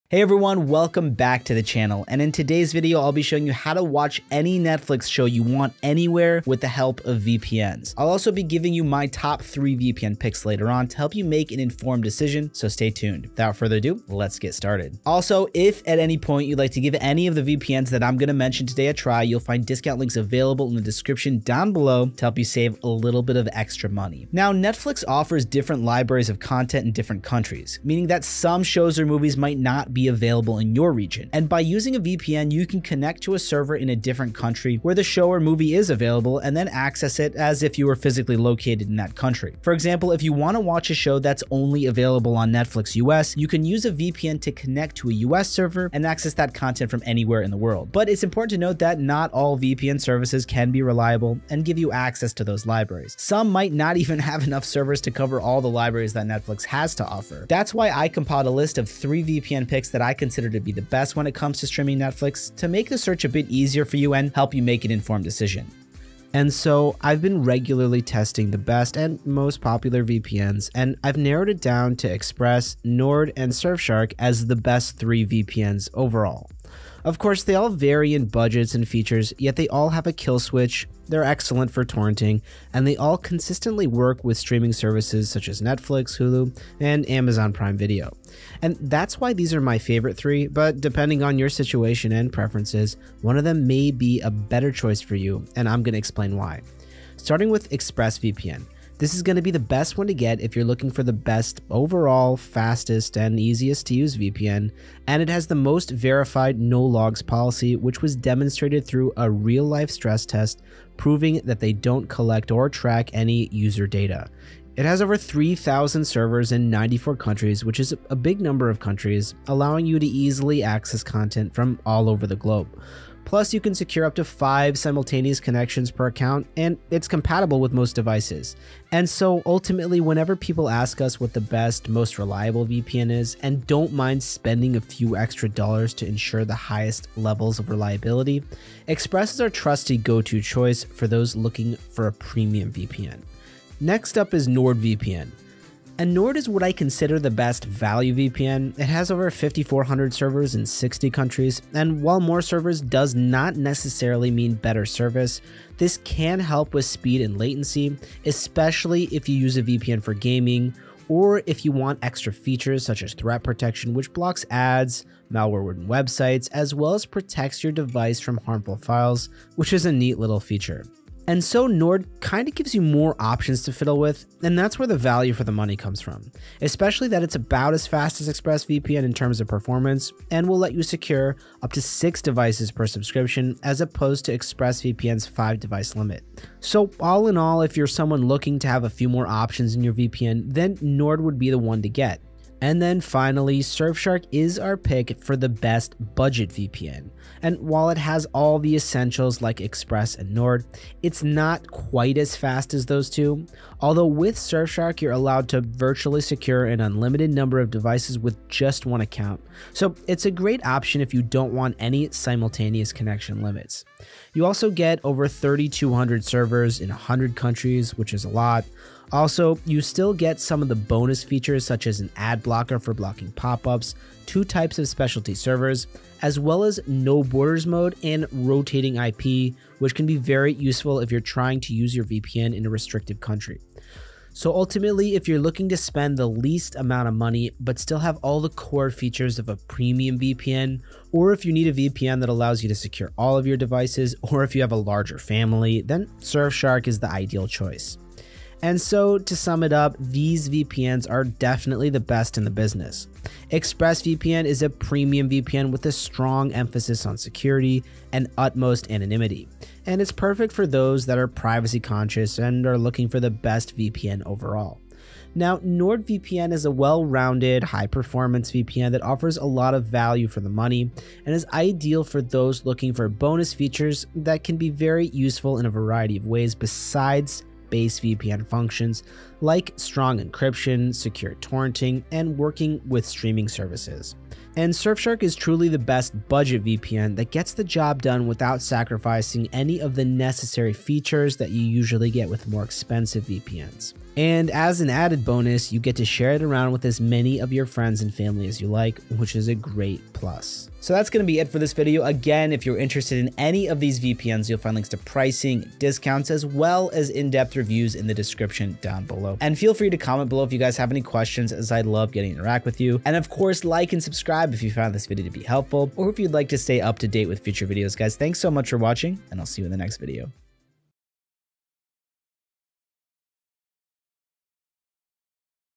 How to Watch Netflix Anywhere With A VPN
How to watch Netflix anywhere with a vpn. Unlock Netflix from anywhere with a VPN! Learn how to watch Netflix from any country with a VPN. Follow along as we show you step-by-step.

Hey everyone, welcome back. And in today’s video, I’ll be showing you how to watch any Netflix show you want anywhere with the help of VPNs. And without further ado, let’s get started. Also, if at any point you’d like to give any of the VPNs that I’m going to mention today a try, you’ll find discount links available in the description down below to help you save some extra money. Now, Netflix offers different libraries of content in different countries, meaning that some shows or movies might not be available in your region. And by using a VPN, you can connect to a server in a different country where the show or movie is available and then access it as if you were physically located in that country. For example, if you want to watch a show that is only available on Netflix US, you can use a VPN to connect to a US server and access that content from anywhere in the world. But it's important to note that not all VPN services can be reliable and give you access to those libraries. Some might not even have enough servers to cover all the libraries that Netflix has to offer. That’s why I compiled a list of 3 VPN picks that I consider the best when it comes to streaming Netflix, to make the search a bit easier for you and help you make an informed decision. So that’s it for this video. Again,  if you’re interested in any of these VPNs, you’ll find links to pricing and discounts, as well as in-depth reviews, in the description down below. And feel free to comment below if you have any questions, as I love getting to interact with you guys, and like and subscribe if you found this video to be helpful or would like to stay up to date with future videos!

Hope you enjoyed my How To Watch Netflix Anywhere With A VPN Video.